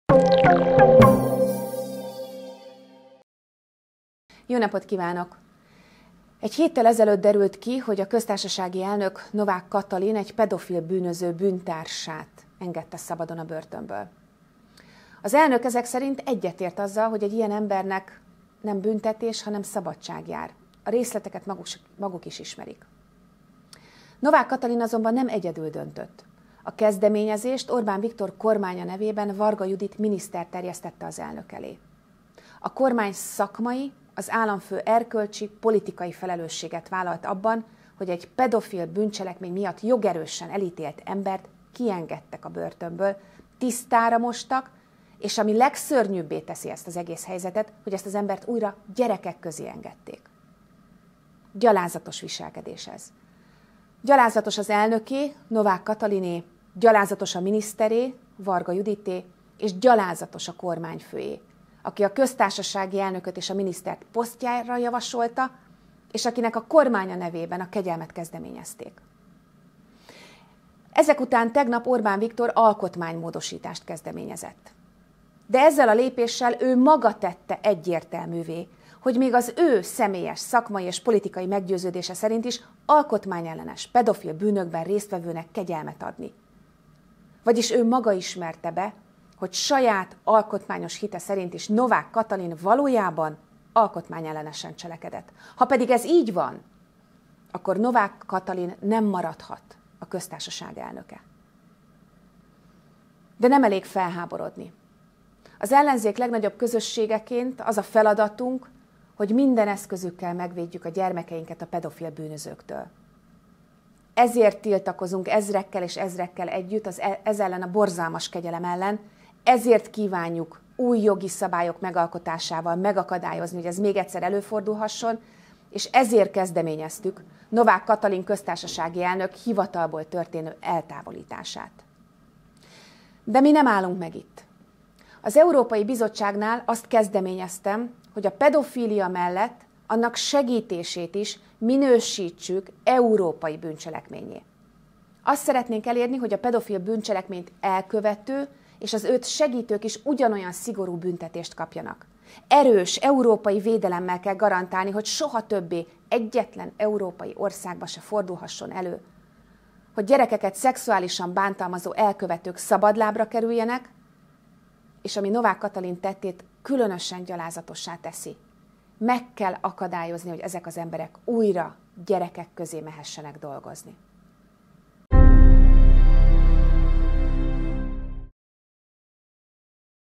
Európai jogalkotást kezdeményezünk a pedofilbűncselekmények kapcsán
BEJELENTÉS: Európai jogalkotást kezdeményezünk a pedofilbűncselekmények kapcsán.

Nem elég felháborodni. Az ellenzék legnagyobb közösségeként az a feladatunk, hogy minden eszközünkkel megvédjük a gyermekeinket a pedofil bűnözőktől.